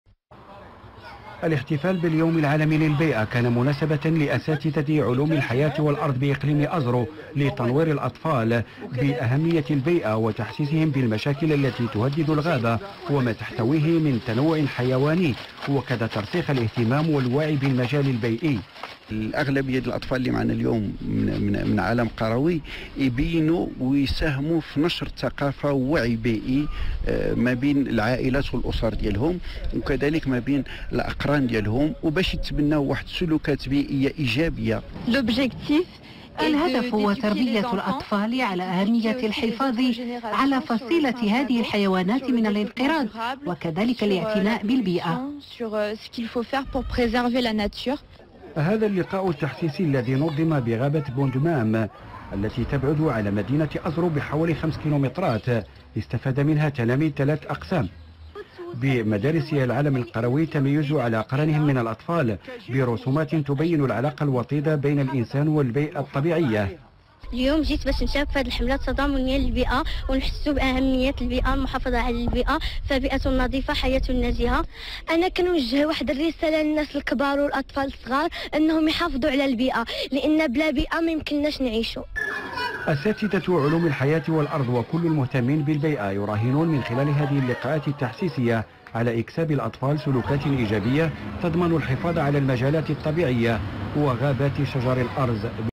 Moroccan Primate Conservation
In Moroccan Arabic! This video is a press clip from our latest education programme in Morocco. Enjoy! (Please note that we strongly discourage people from feeding Barbary macaques in the wild. This humanises them and allows their capture and leaves them at risk from being run over on the road. The man feeding a macaque at the end is not our footage and is an example of what not to do!)